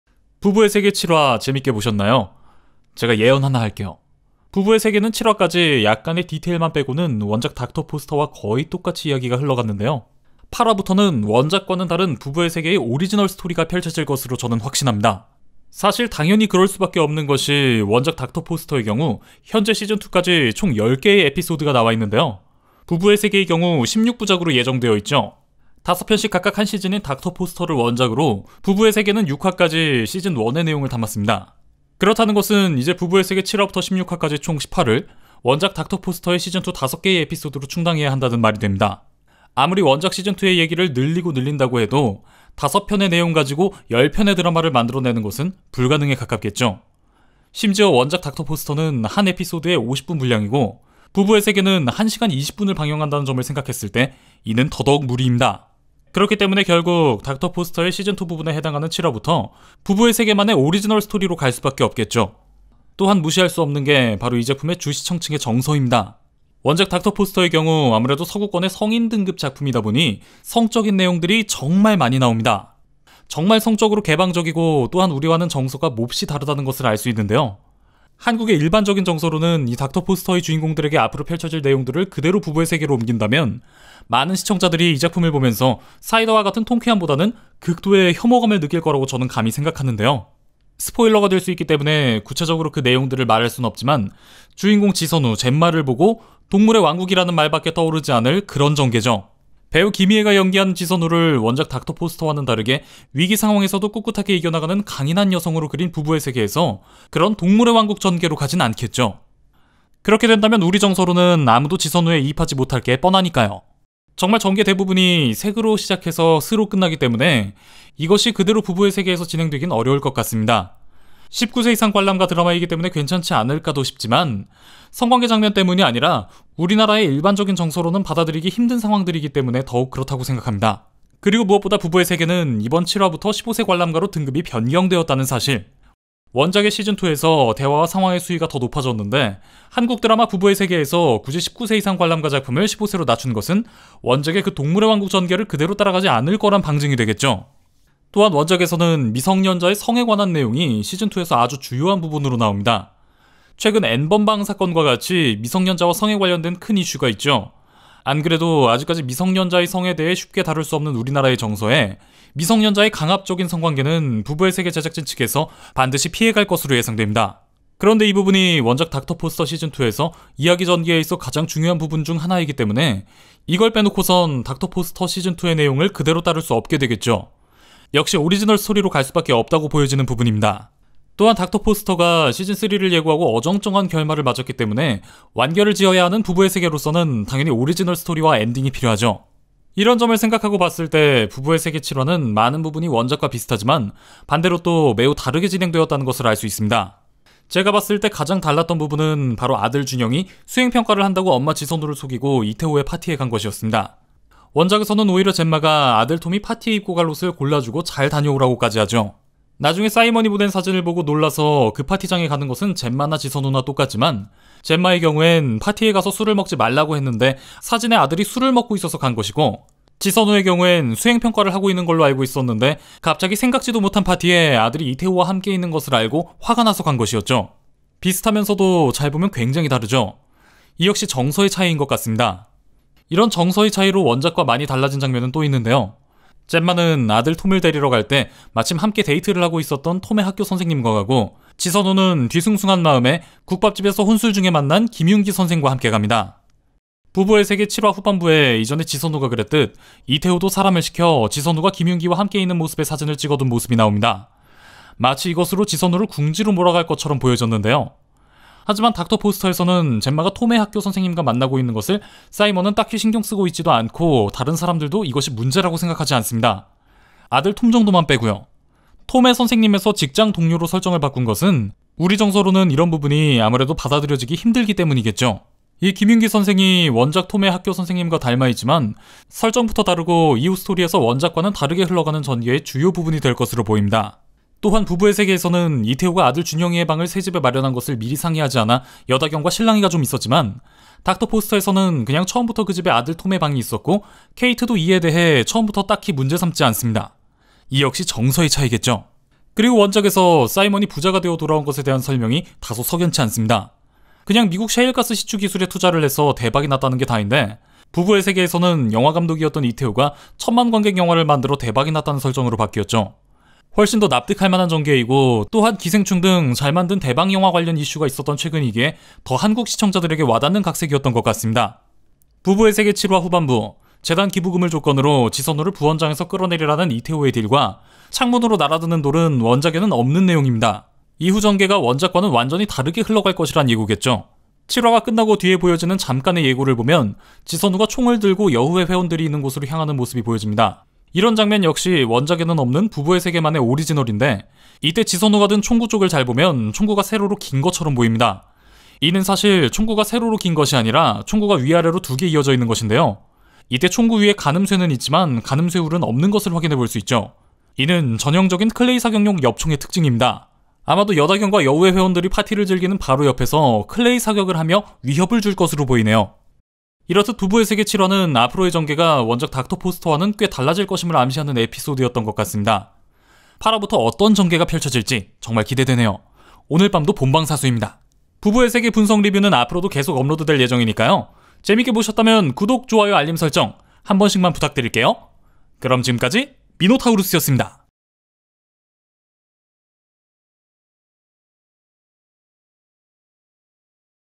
《부부의 세계》 8화부터 더 재밌을 수밖에 없는 이유
부부의 세계 8화부터는 원작 닥터 포스터와는 다른 이야기로 진행된다?
부부의 세계 7화 분석을 통해 보는, 부부의 세계 8화부터 원작과 다른 전개로 진행될 수밖에 없는 이유.